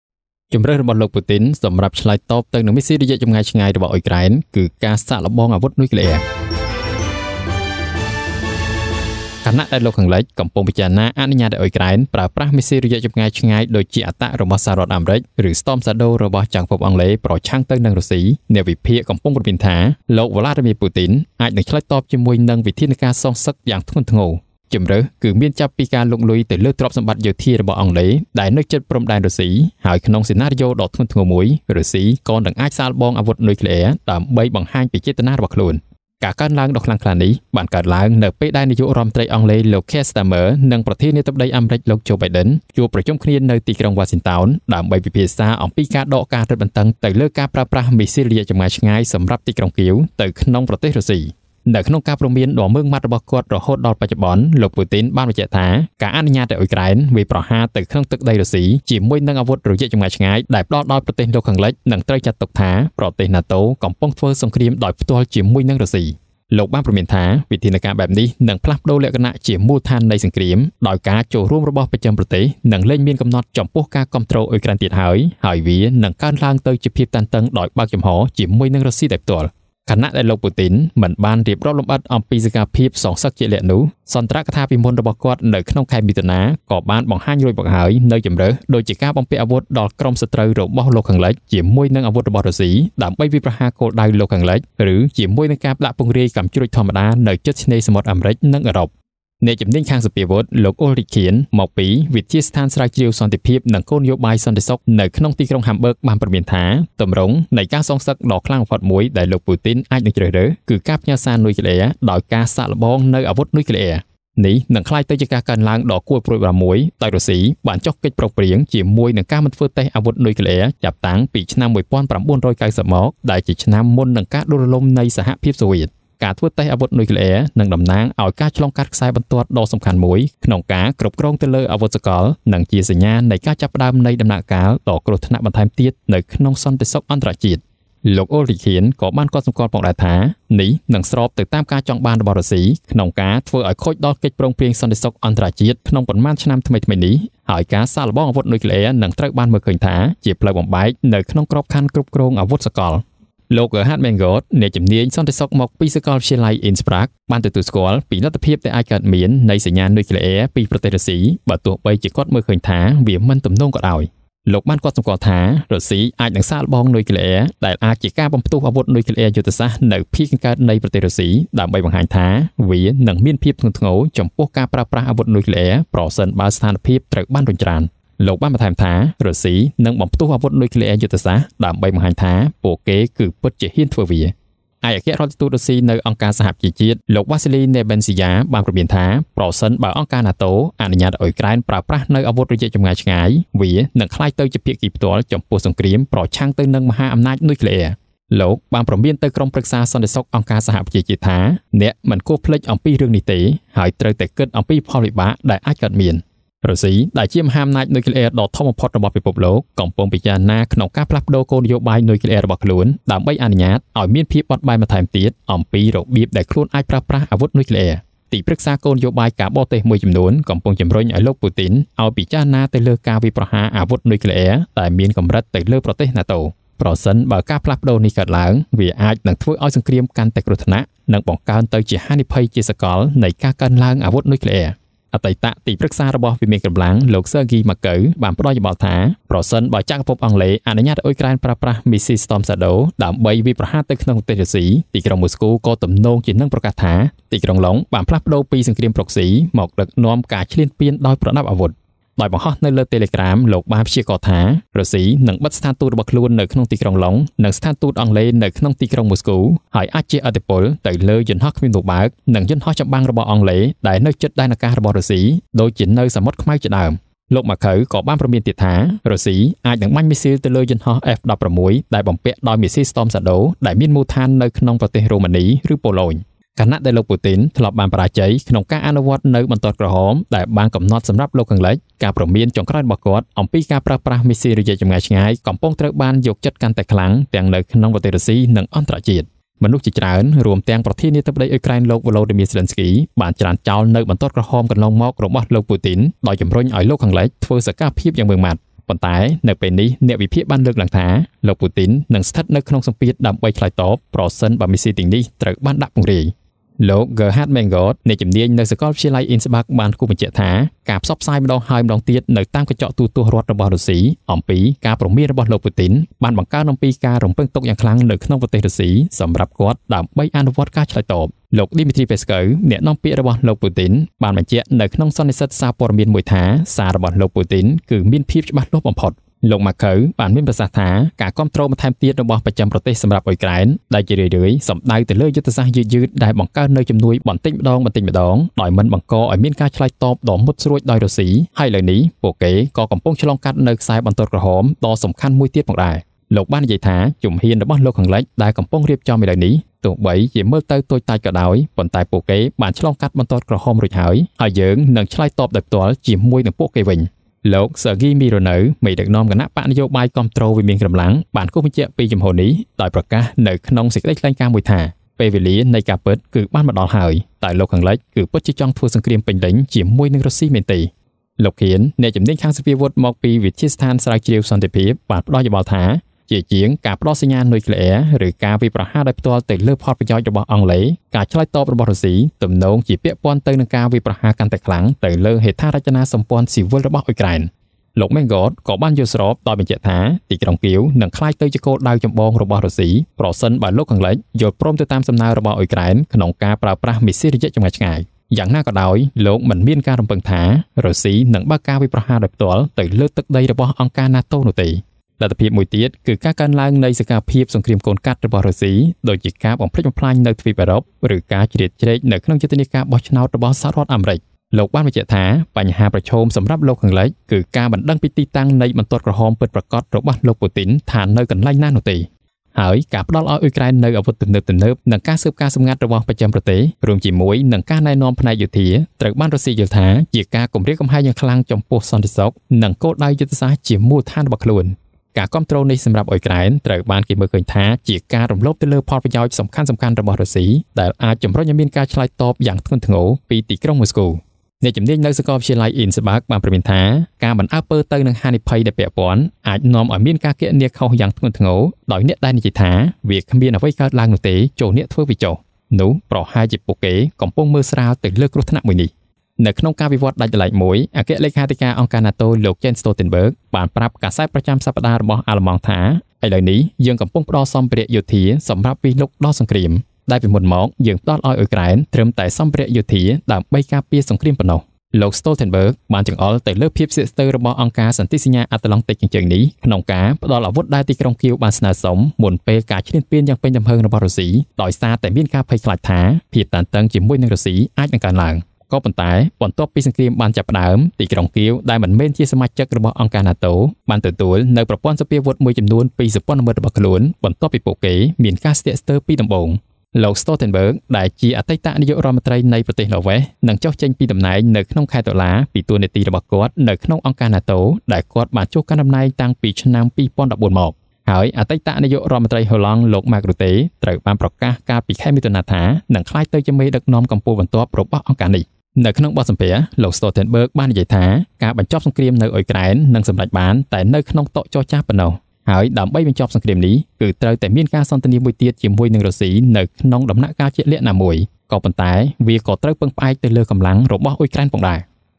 ជម្រើសរបស់លោកពូទីន សម្រាប់ឆ្លើយតបនឹងមីស៊ីលរយៈចម្ងាយឆ្ងាយអ៊ុយក្រែន គឺការសាកល្បងអាវុធនុយក្លេអ៊ែរ
អតីតទីប្រឹក្សារបស់វិមានក្រឹមឡាំង លោក Sergei Markov បានផ្តល់យោបល់ថា ប្រសិនបើចក្រភពអង់គ្លេសអនុញ្ញាតឱ្យអ៊ុយក្រែនប្រើប្រាស់មីស៊ីល Storm Shadow ដើម្បីវាយប្រហារប្រទេសរុស្ស៊ី ទីក្រុងម៉ូស្គូទំនងជានឹងប្រកាសថា ទីក្រុងឡុងដ៍បានផ្លាស់ប្តូរពីសង្រ្គាមប្រូកស៊ី មកដឹកនាំការឈ្លានពានដោយ ប្រដាប់អាវុធ។ ដោយបង្ហោះនៅលើ Telegram លោកបានព្យាករណ៍ថា រុស្ស៊ីនឹងបិទស្ថានទូតរបស់ខ្លួននៅទីក្រុងឡុងដ៍ និងស្ថានទូតអង់គ្លេសនៅទីក្រុងមូស្គូ ហើយអាចជះឥទ្ធិពលទៅលើយន្តហោះគ្មានមនុស្សបើក និងយន្តហោះចម្បាំងរបស់អង់គ្លេសដែលនៅជិតដែនអាកាសរុស្ស៊ី ដូចជានៅសមុទ្រខ្មៅជាដើម។ លោក Markov ក៏បានព្រមានថា រុស្ស៊ីអាចនឹងបាញ់មីស៊ីលទៅលើយន្តហោះ F-16 ដែលបំពាក់ដោយមីស៊ីល Storm Shadows ដែលមានមូលដ្ឋាននៅក្នុង ប្រទេសរ៉ូម៉ានី ឬប៉ូឡូញ។